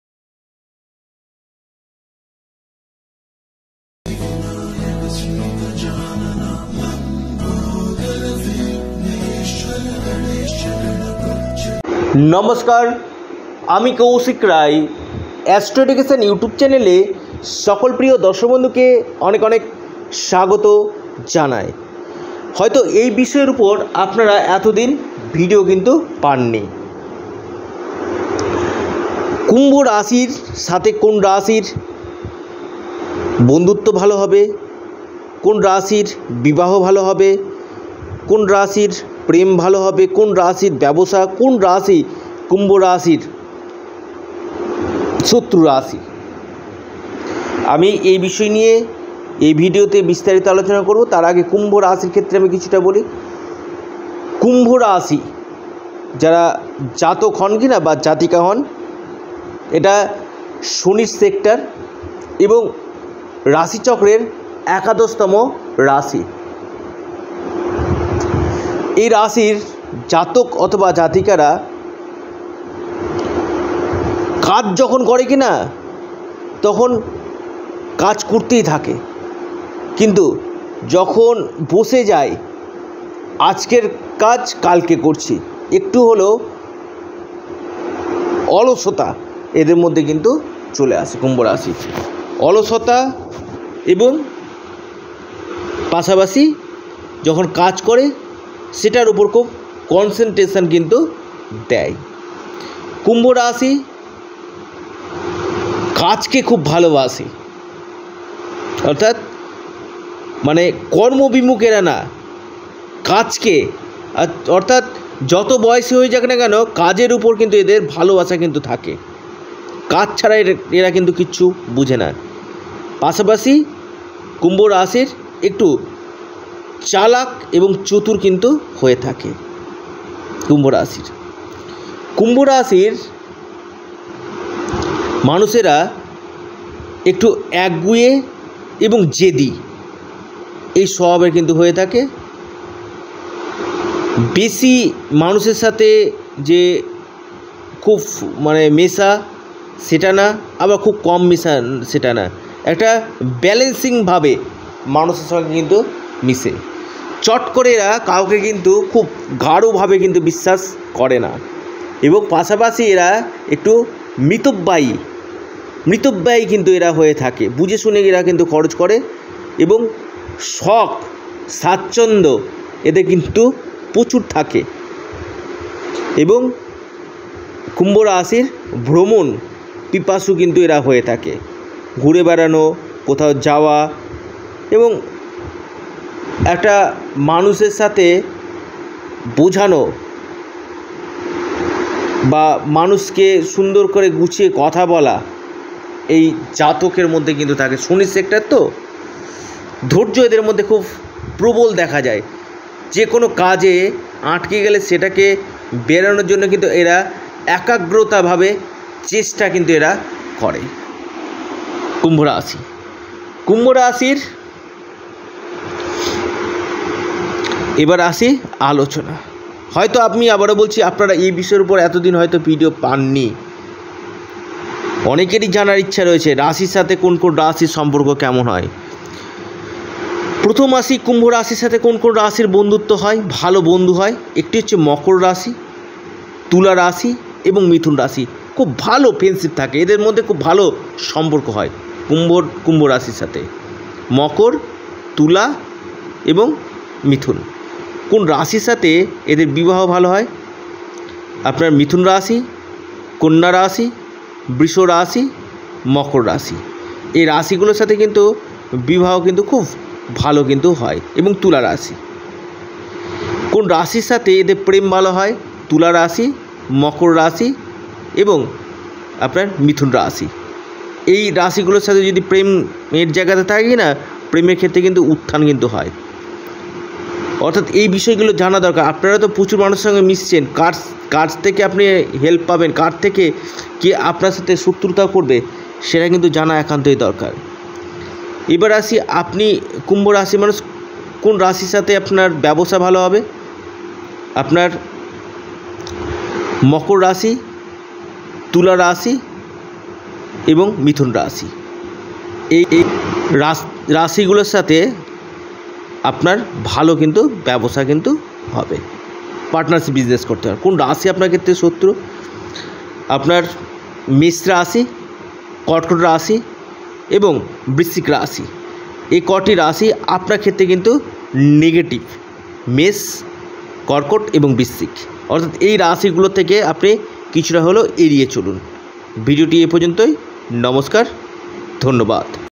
কুম্ভ রাশি পূর্ণাঙ্গ বিশ্লেষণ!
কুম্ভ রাশির সাথে ১১ টি রাশির সম্পর্ক কেমন এই বিষয় নিয়ে এই ভিডিওতে বিস্তারিত আলোচনা করা হয়েছে!